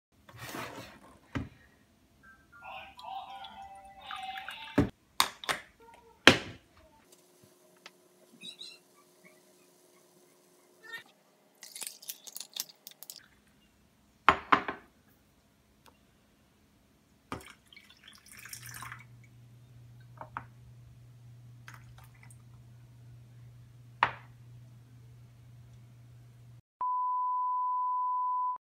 [커피] 오늘의 커피/홈카페/아메리카노/아이스 아메리카노/모카포트/일리 커피/디카페인
오늘은 아이스 아메리카노를 마셨습니다.
아이스 아메리카노가 간절해지는 계절이 왔네요...:))